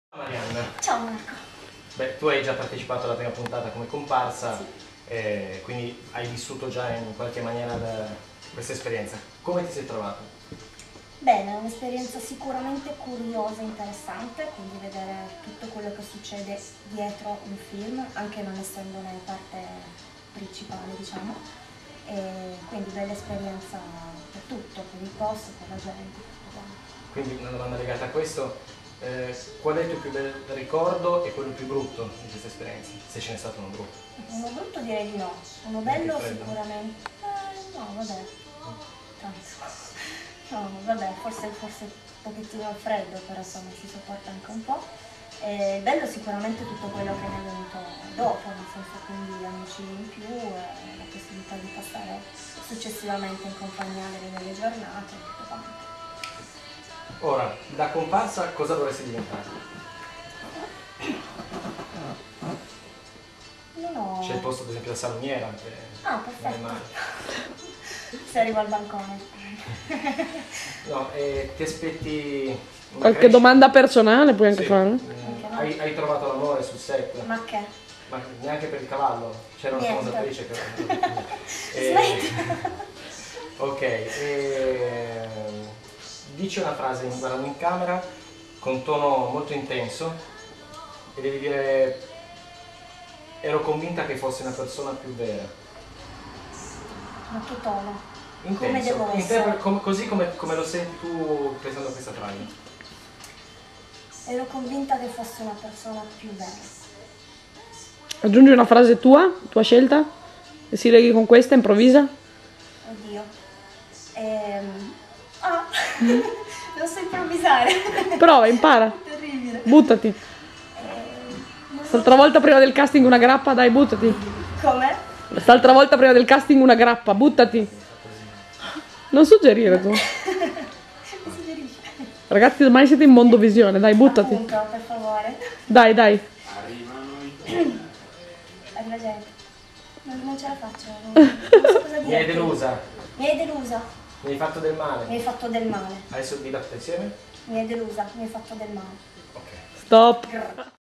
Casting The Wild Side - Marianna Bolla
Casting attori e comparse x la seconda puntata della fiction "the wild side"